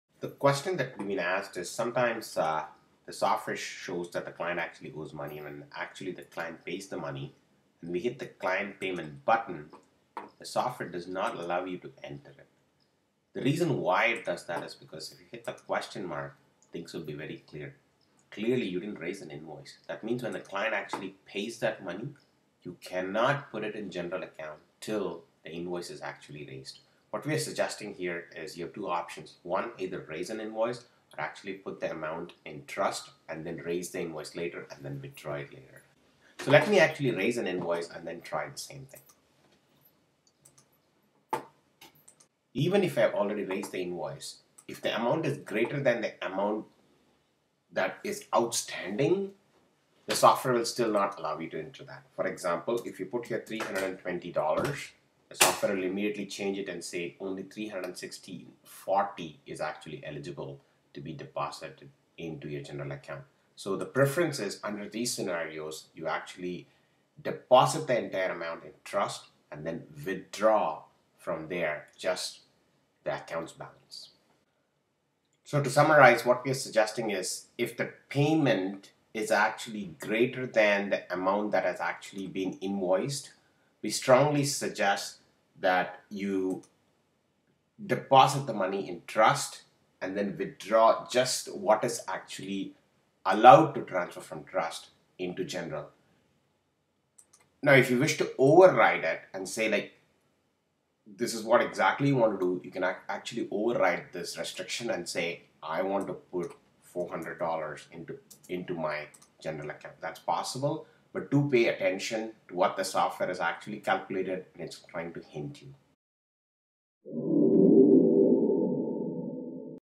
Q114. Why is the client payment amount disabled or Max amount set?
The software calculates the maximum amount that one can receive in General account based on Invoice balance. If the software shows "$0.00" or shows a maximum value that you can actually deposit and if you try and deposit beyond that it tries to put a restriction to hint you that probably you need to raise more invoice to cover for this deposit or actually deposit the money in trust and transfer only the eligible amount.